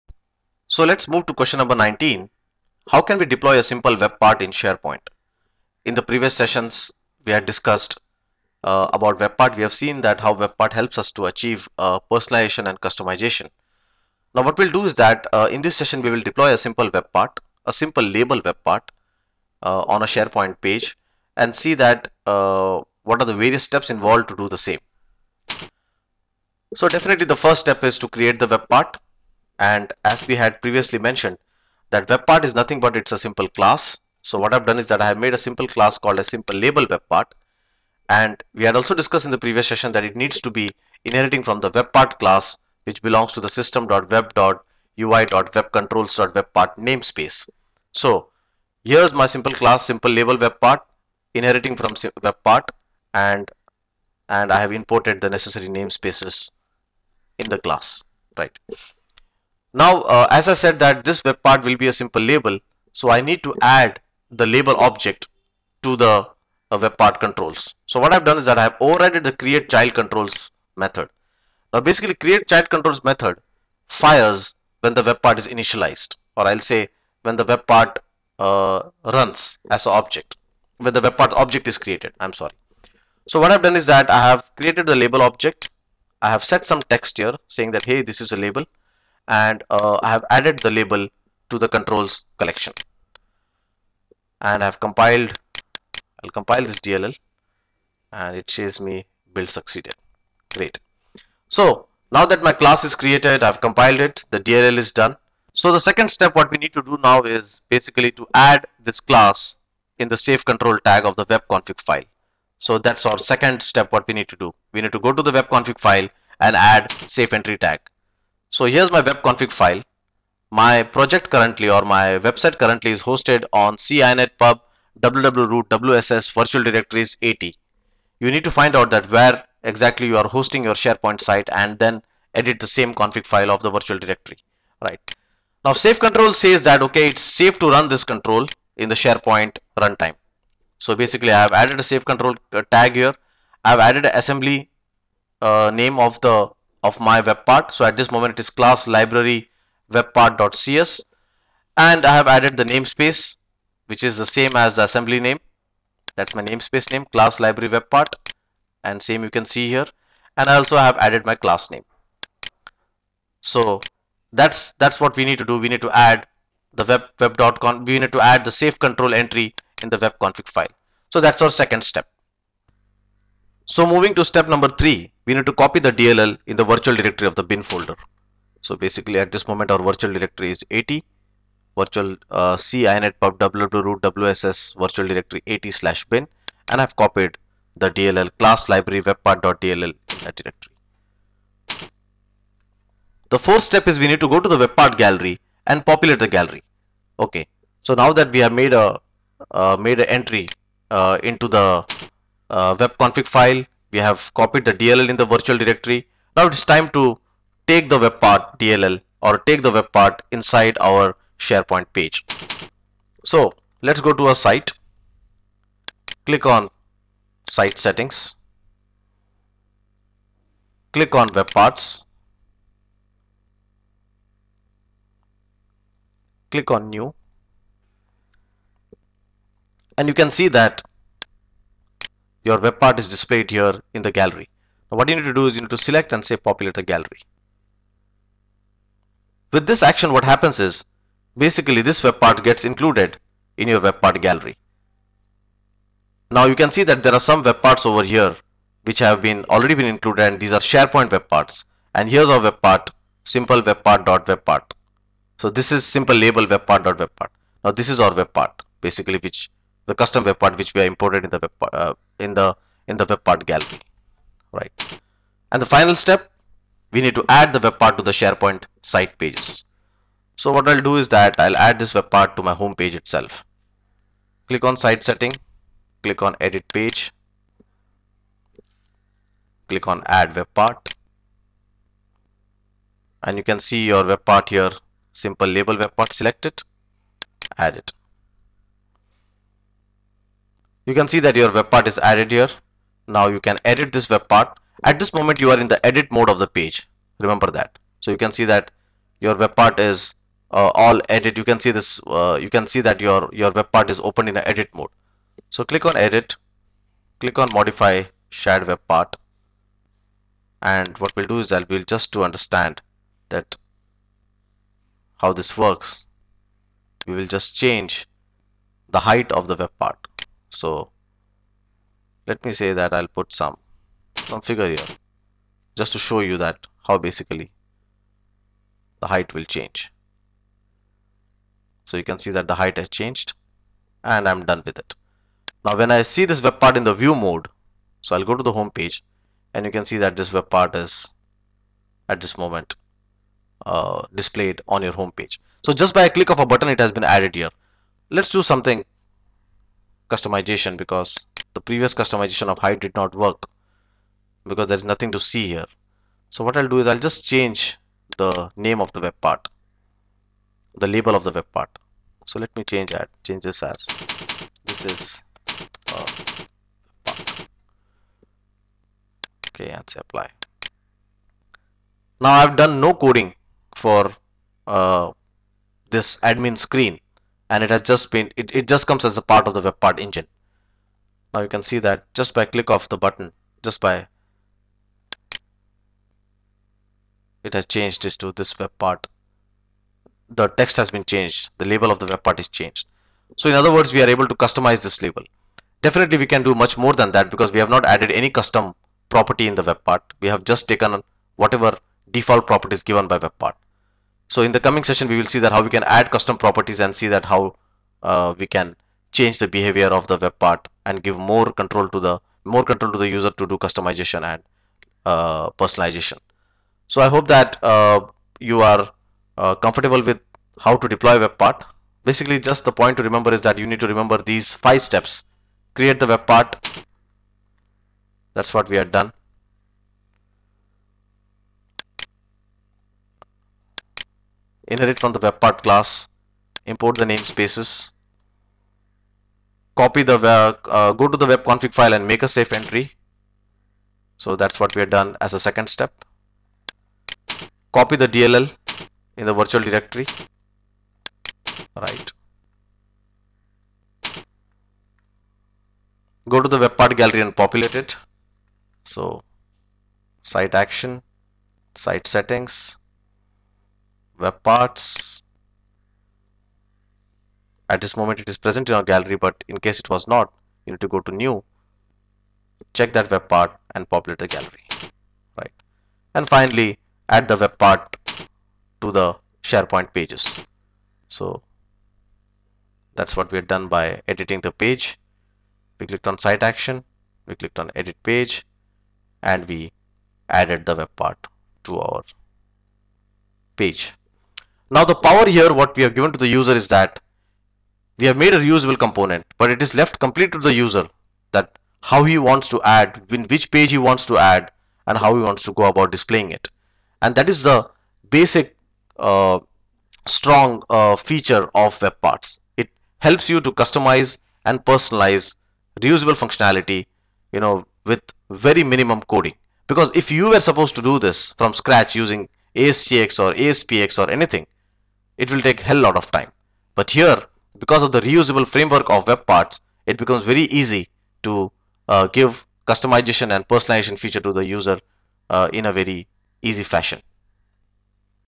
Learn SharePoint Step By Step:- How can we deploy a simple Webpart in SharePoint?
In this video we will learn How can we deploy a simple Webpart in SharePoint?

For more videos on Sharepoint and other .NET technologies log